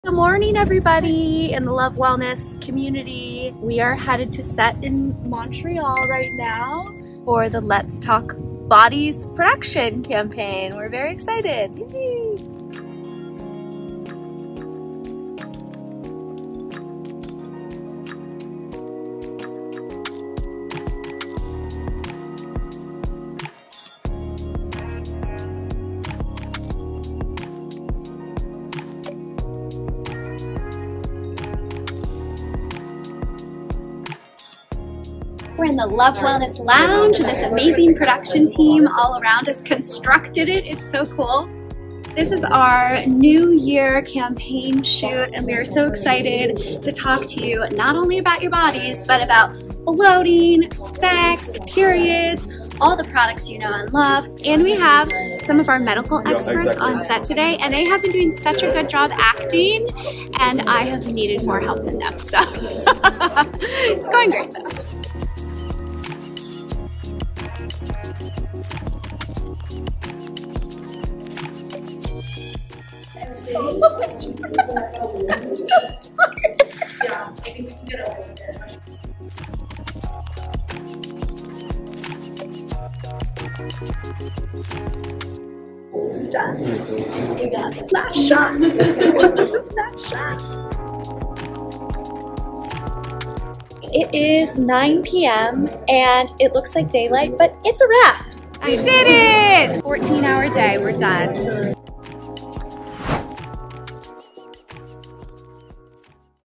Go Behind-the-Scenes with Love Wellness Founder and CEO, Lo Bosworth
In honor of International Women's Day, join Love Wellness CEO and Founder, Lo Bosworth, behind-the-scenes at our "Let's Talk Bodies" video shoot! See all that happens behind-the-scenes and get to know Lo and our wellness experts!